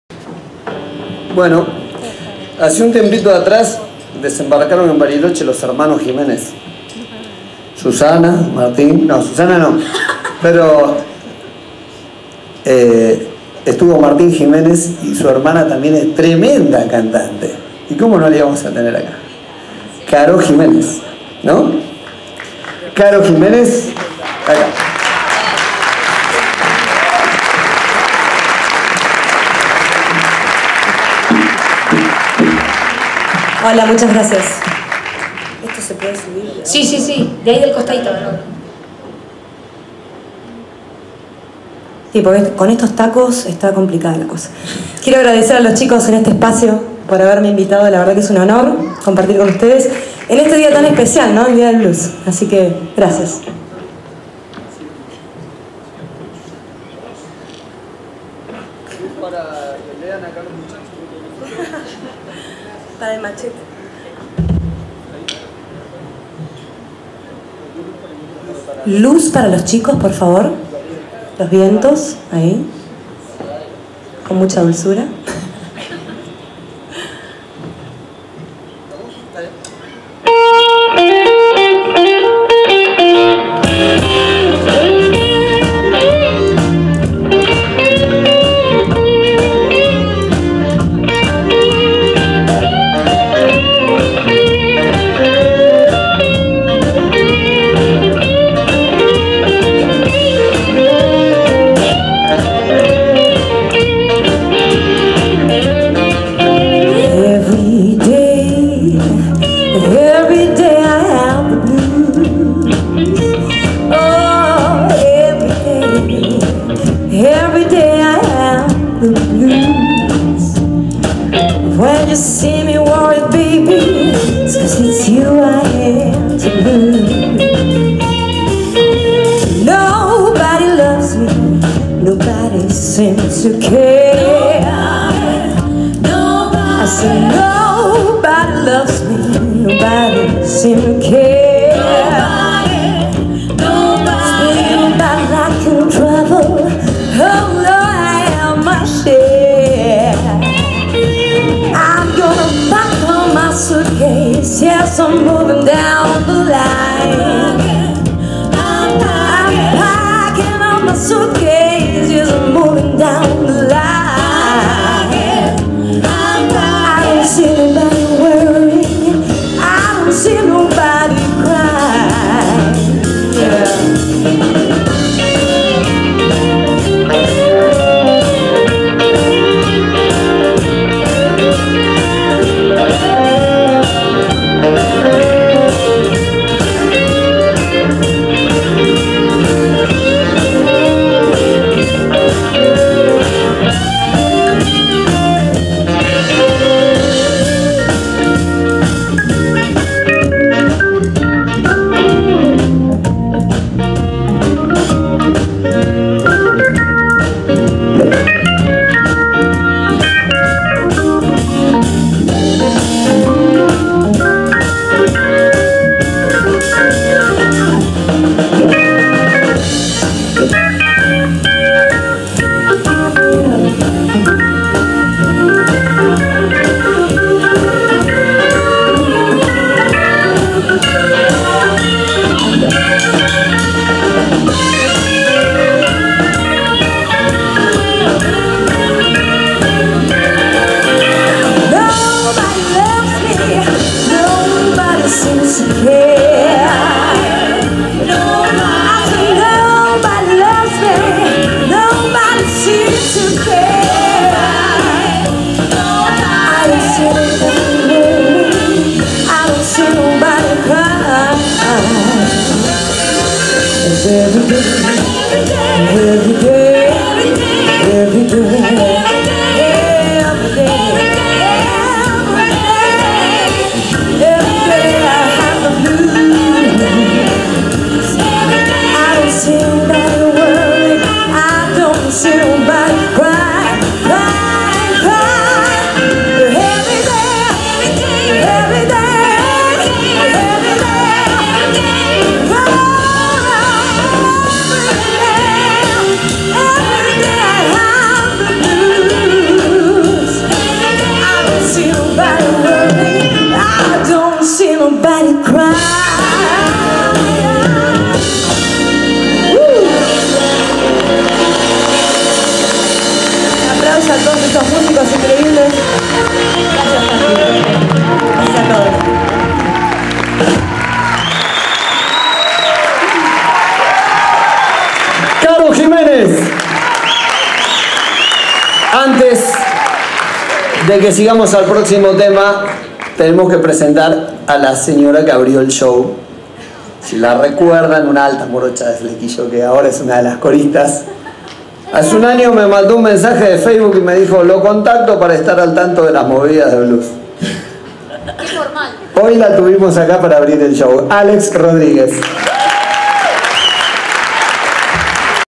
ROCANBLUS - La Baita DIB 1-8-15 (18)
En Bariloche, Teatro La Baita, Día Internacional del Blues.
El Alambique con muchos de los grandes músicos de nuestra ciudad.
Un orgullo para todos los que amamos la música y el blues en particular.
Una noche inolvidable que quedará por mucho tiempo en el recuerdo de todos.